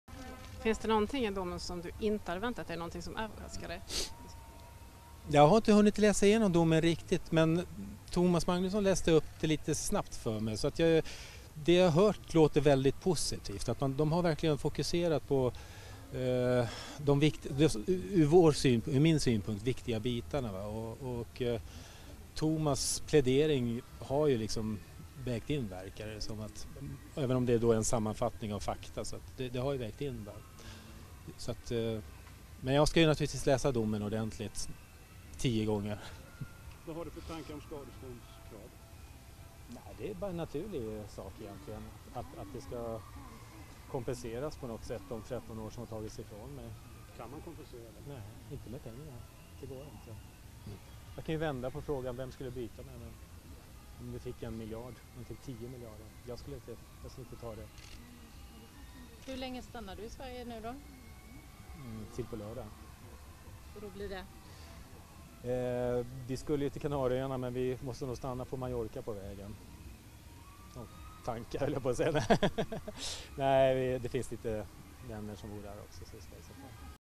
Kaj Linna: "Jag ska läsa domen tio gånger"
Efter att ha suttit frihetsberövad i 13 år beviljades han resning och släpptes på fri fot. Nu har hovrättens dom fallit: Kaj Linna frias helt.